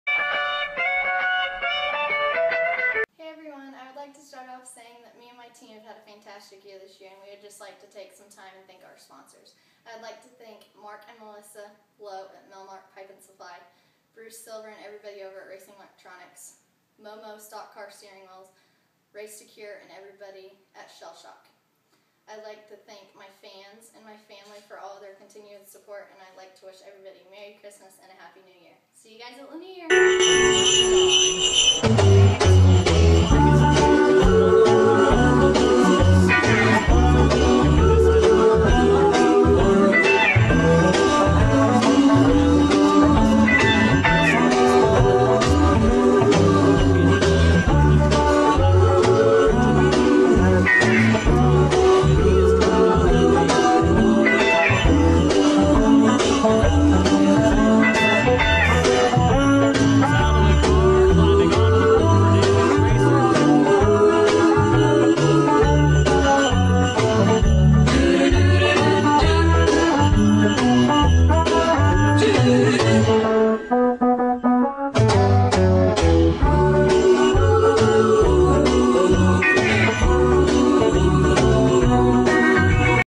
Merry Christmas From Kenzie Ruston Racing
Kenzie Ruston would like to thank everyone for a great 2011 season and wish everyone a very happy holidays